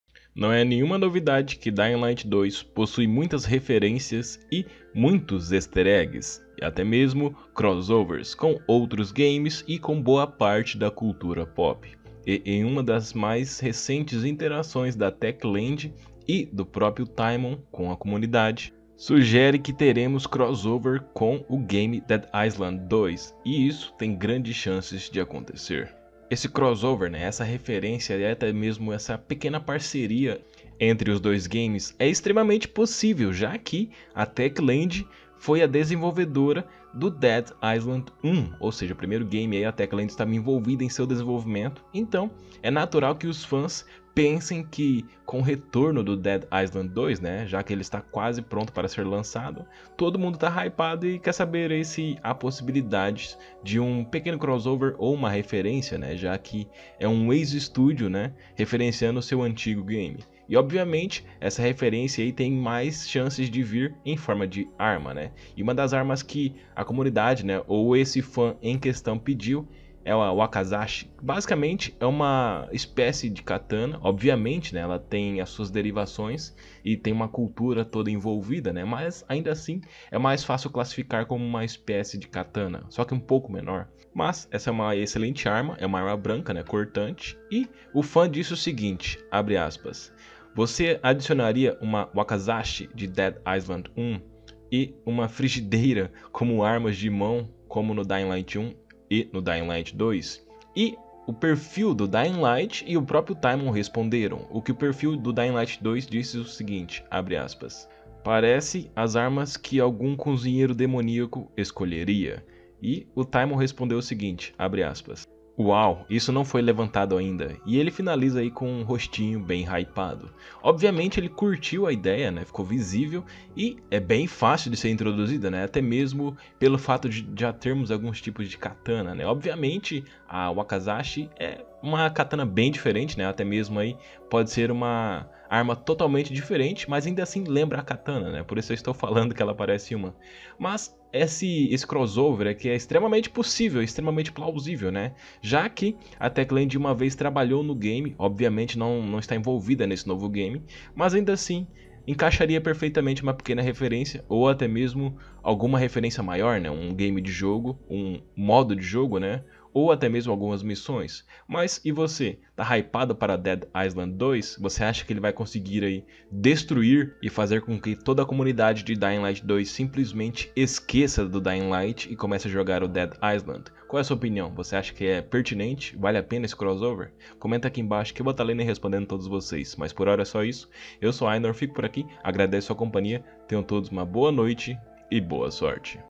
DYING LIGHT 2 TEREMOS CROSSOVER COM DEAD ISLAND 2 PS4/XBOX/PC
Não é nenhuma novidade que dying light 2 possui muitas referencias e muitos easter eggs até mesmo crossovers com outros games e com boa parte da cultura e em uma das mais recentes interações da Techland e do proprio tymon com a comunidade sugere  que teremos um crossover com o game Dead island 2 e isso tem grandes chances de acontecer .

Videos todos os dias 19:30  e
11:30